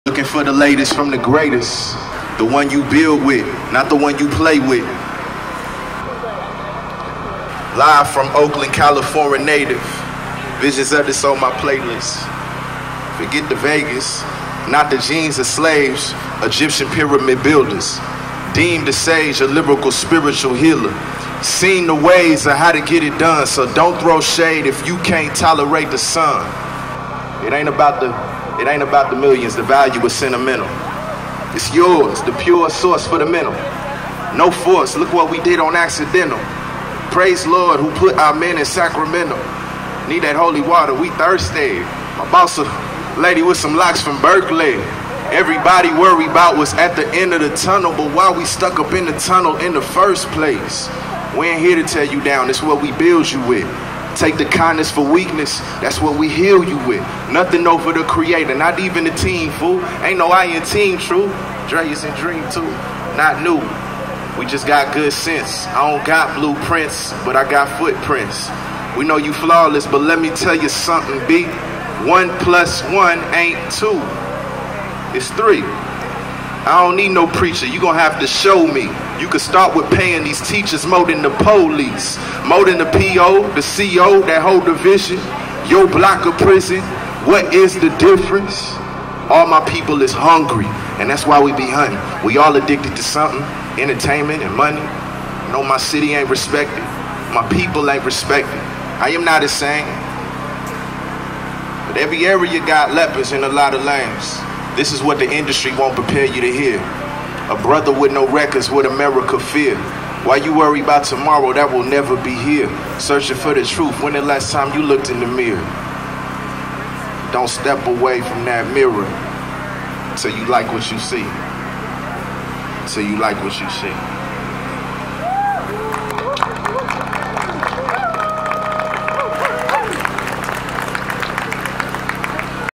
( DSCN1322 ) 8th Annual Oscar Grant Vigil ~ Pt.16
8th Annual Oscar Grant Vigil — Gone But Not Forgotten
@Grant (Fruitvale) BART station, Oakland